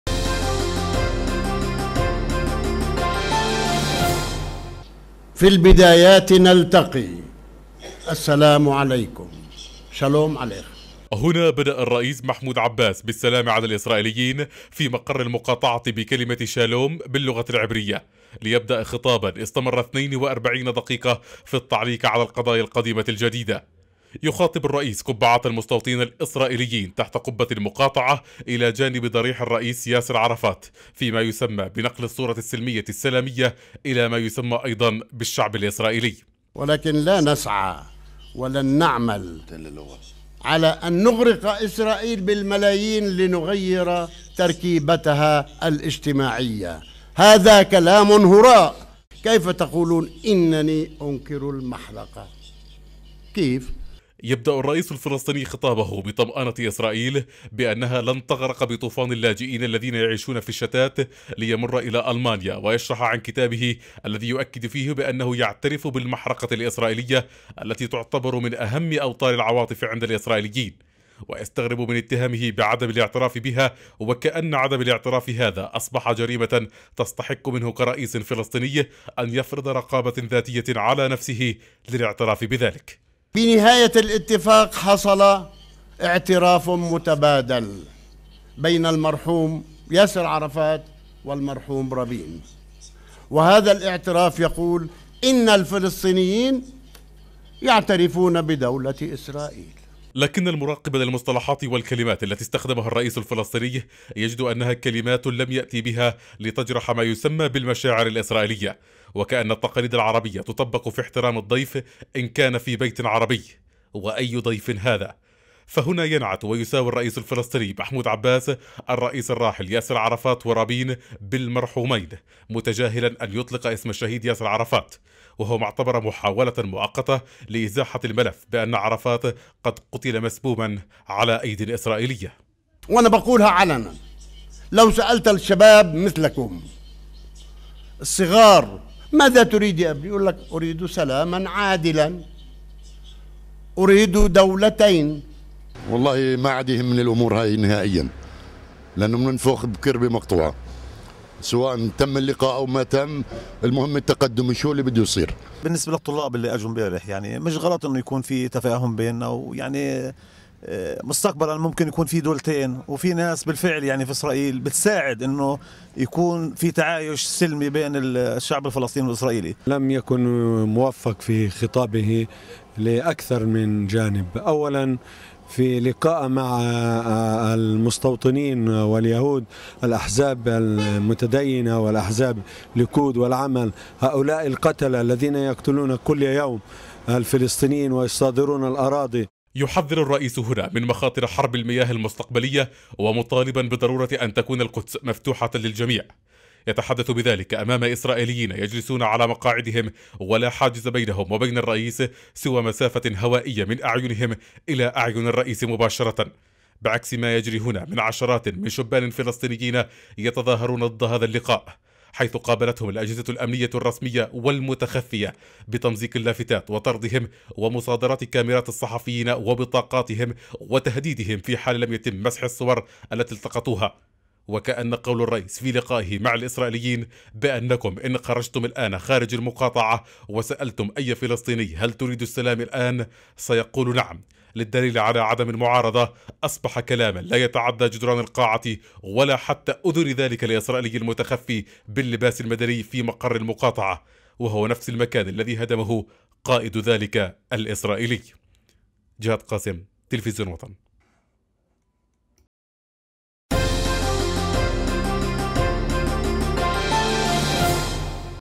الرئيس يخطب في عشرات الإسرائيليين بينهم "جنود احتياط"
وطن للأنباء - جهاد قاسم: افتتح الرئيس الفلسطيني محمود عباس لقاءه مع 170 طالبًا إسرائيليًا في مقر الرئاسة (المقاطعة)  بكلمة "شالوم" باللغة العبرية، التي تعني السلام.

وتم اقتباس الكلمة على مواقع التواصل الاجتماعي وسط حالة من السخط لاستخدامها، وفق مراقبين.

وقال الرئيس عباس في خطابه، إنه "لا يريد إغراق إسرائيل بملايين اللاجئين الفلسطينيين".

مضيفًا أن "الدعاية التي تروج عن الرئاسة الفلسطينية بأنها تريد إعادة خمسة ملايين لاجئ فلسطيني لتدمير إلى دولة إسرائيل لتدميرها".

وحول "المحرقة الإسرائيلية"، قال الرئيس عباس إنه "يعترف بها، وراح ضحيتها ملايين الإسرائيليين".

وقال إن " الفلسطينيين يريدون السلام، فلو سئل أي فلسطيني (ماذا تريد؟) ستكون إجابته بالتأكيد (السلام وحل الدولتين)".
يشار إلى أن قسمًا من الطلبة الإسرائيليين الذين استقبلهم الرئيس عباس، جنود احتياط في جيش الاحتلال.